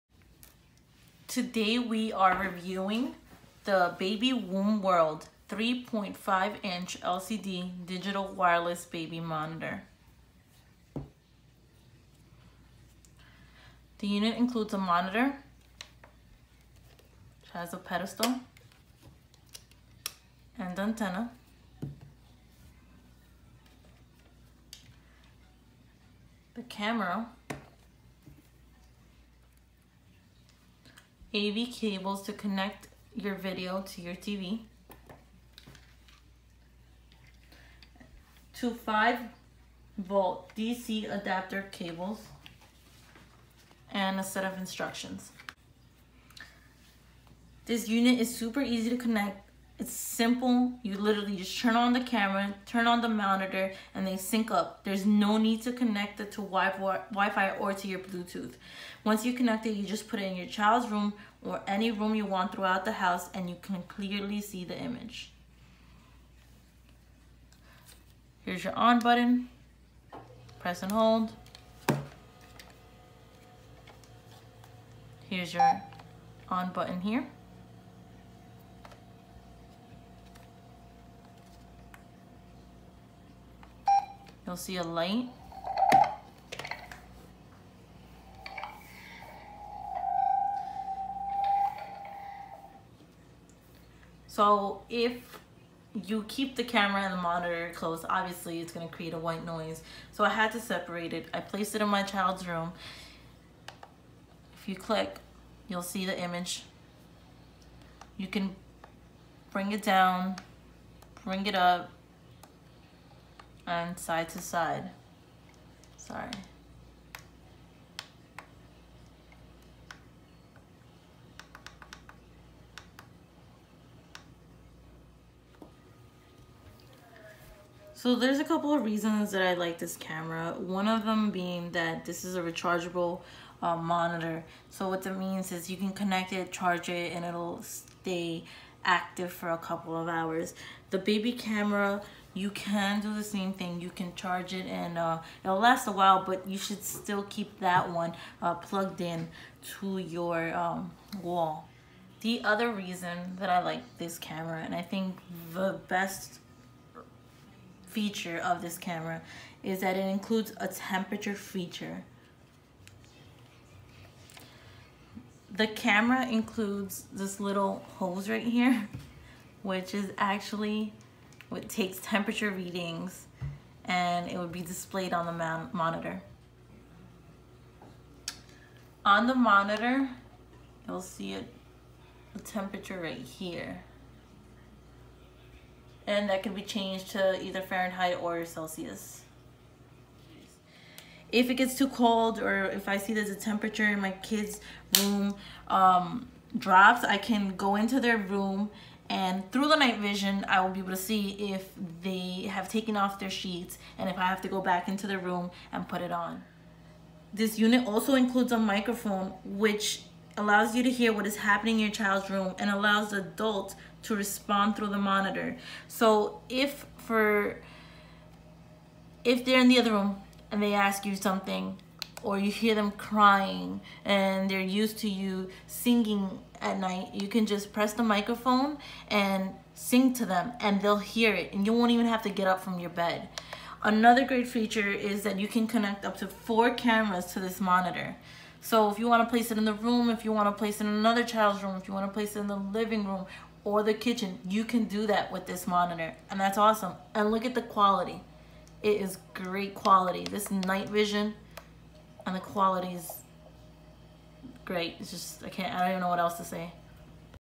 BabyWombWorld LCD digital Wireless Monitor
This is my unbiased review of the BabyWomb World wireless monitor.

This product can be found using the following